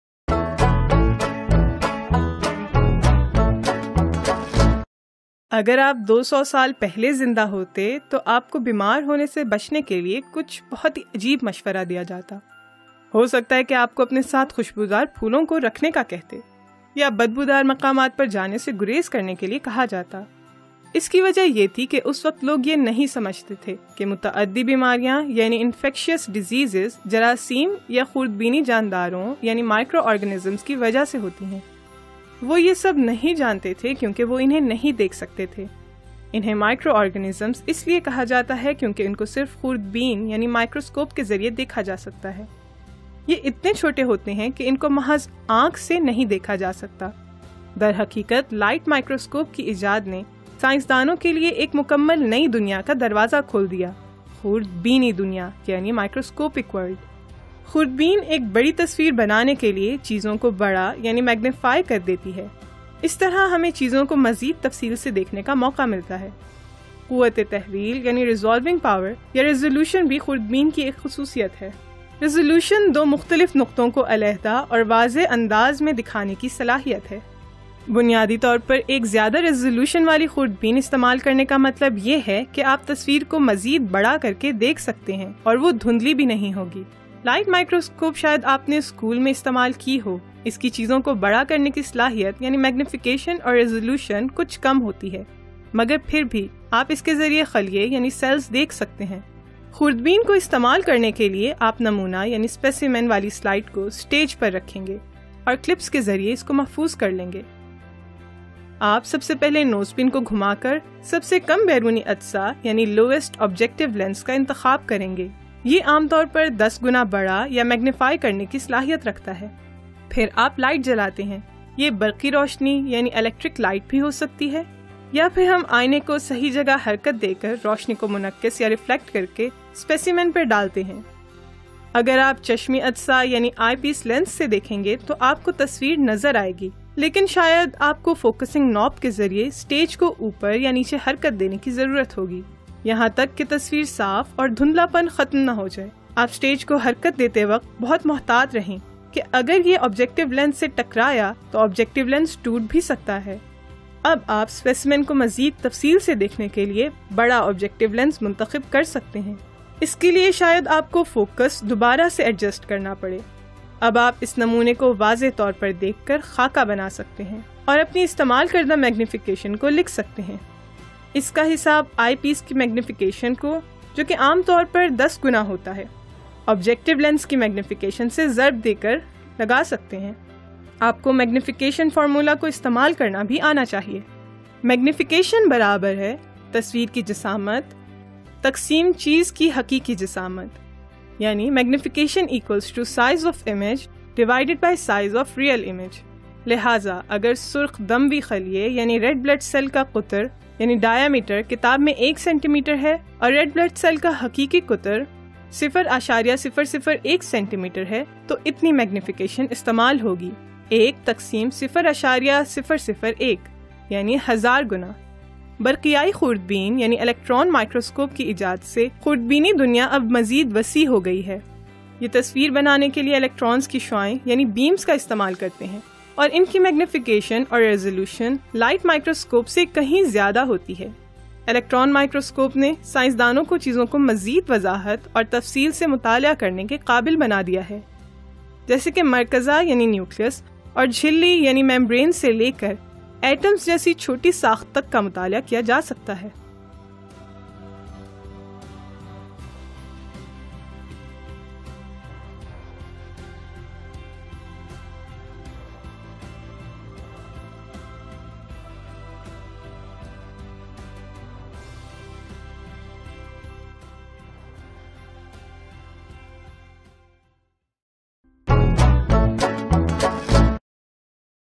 How to use a Microscope | Cells | Biology | Knowledge Platform
There is a lot of living and non-living things around us that cannot be seen through naked eyes. The invention of the microscope opened up a whole new world to scientists - the microscopic world. A microscope magnifies objects to make a larger image. This allows us to see objects in more detail.

This video was created by FuseSchool. If you enjoyed it and want to explore other exciting video lessons, please go